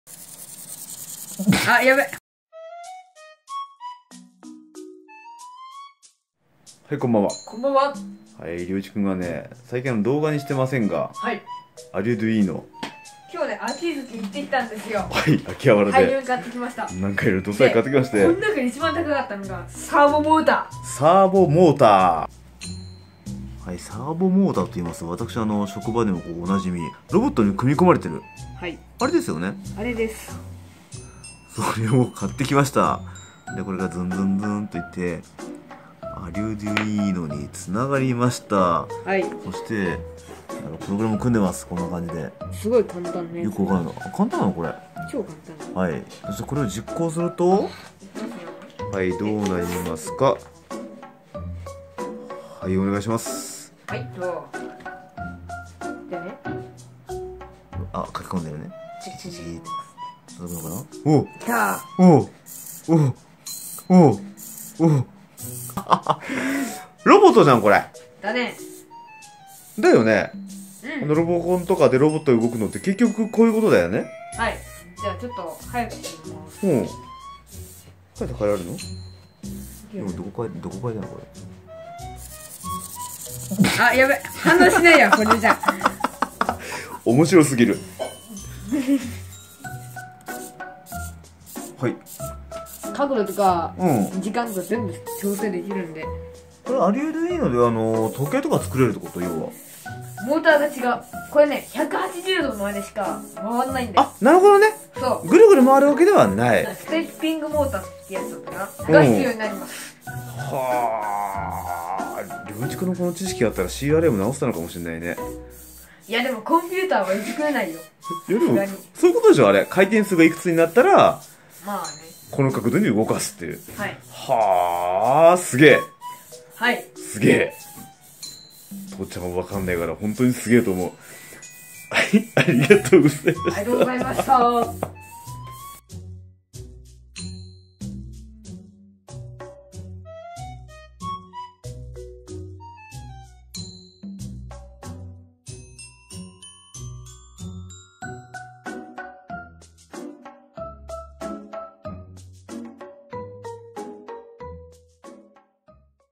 【今日のりょういち】秋葉原の秋月電子で サーボモーター買ってきました（アルデュイーノ 電子工作）
（アルデュイーノ,Arduino,マイコン,プログラミング）
★Arduino(アルデュイーノ)
【今日のリョウイチ】サーボモーターを買ってきて、遊んでおります（笑）

  ◎郵便物の受け取り窓口ができました！　↓↓＼(≧∀≦)／
　  〒106-6137
　  東京都港区六本木6-10-1 六本木ヒルズ森タワー37F（South）
　  UUUM株式会社　　DIY道楽 テツ 宛